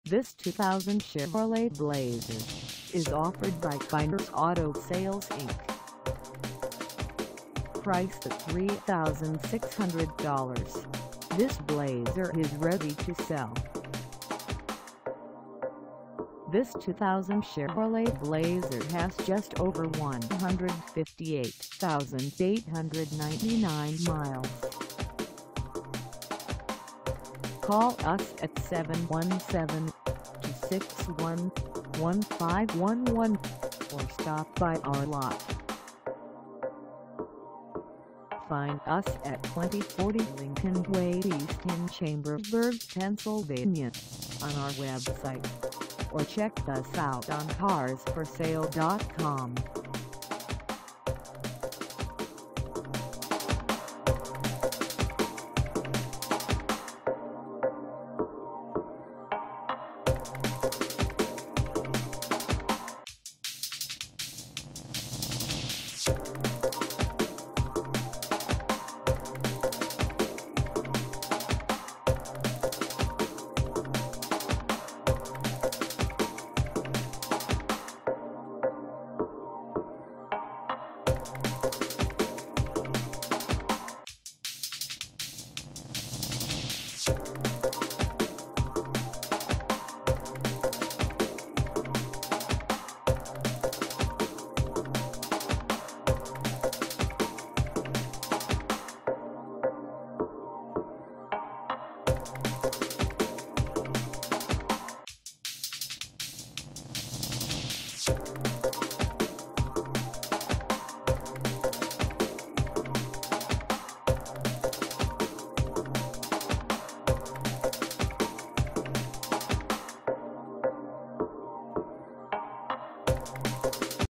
2000 Chevrolet Blazer LS 2dr 4WD SUV for sale in Chambersbur
This 2000 Chevrolet Blazer LS 2dr 4WD SUV is for sale in Chambersburg, PA 17202 at Kyners Auto Sales INC.
Engine: 6-Cylinder 4.3L
Transmission: Unspecified
Color: Beige
Mileage: 158899
Contact Kyners Auto Sales INC 2040 Lincoln Way E Chambersburg, PA 17202 to test drive this 2000 Chevrolet Blazer LS 2dr 4WD SUV today.